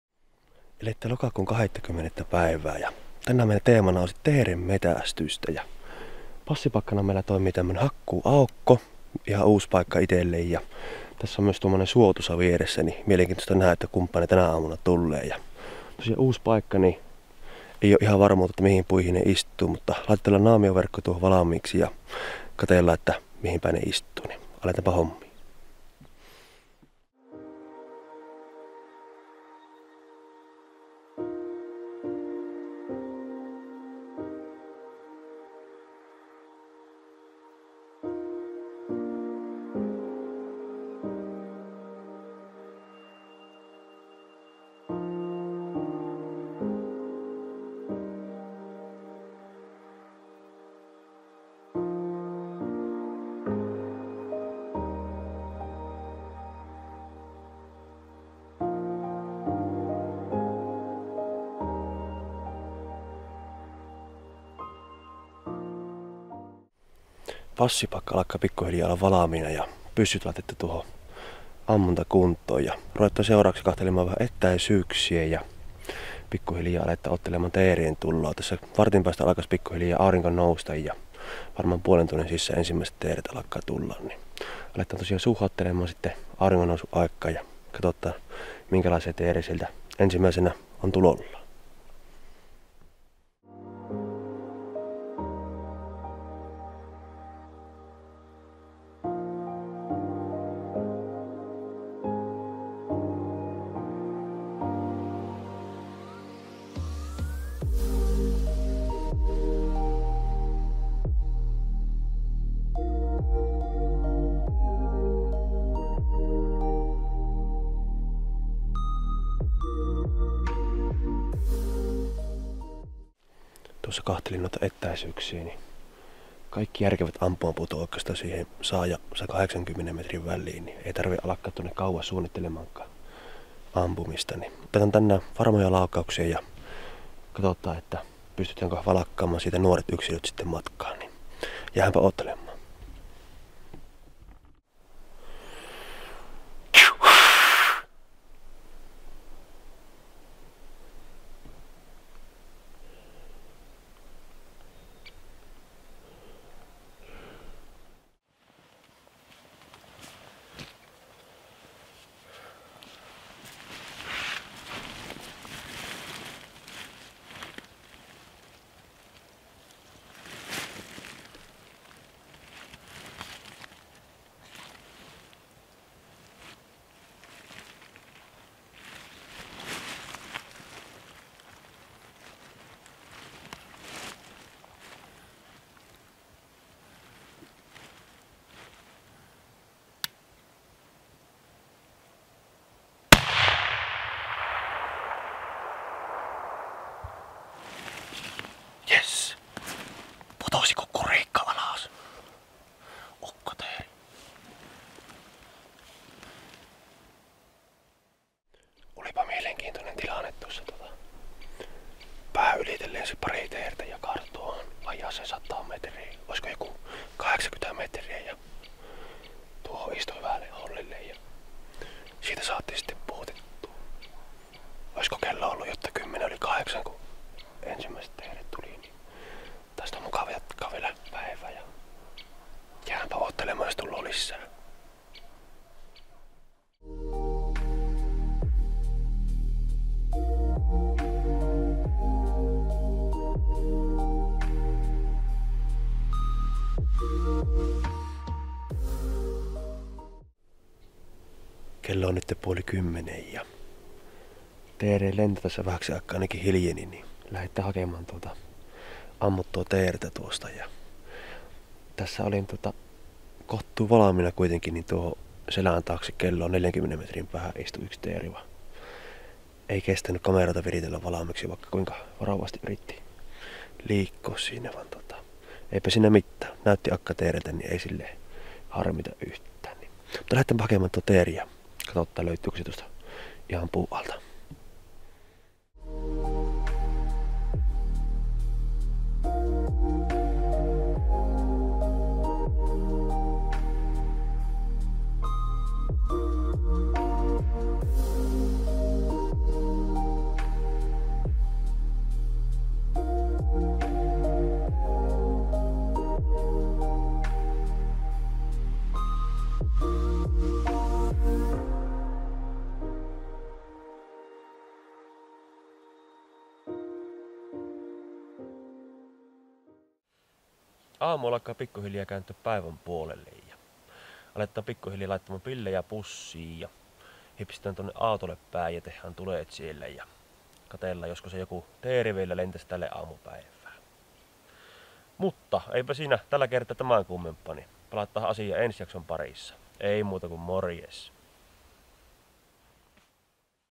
Kanalinnun metsästystä 2022 | Lokakuun teeri | Erämaiden Kulkijat
Tällä kertaa lähdimme metsästämään teeriä uudelle alueelle ja toivoimme yöllä käyneen pikku pakkasen aktivoineen teeret hyviä riistalaukauksia varten.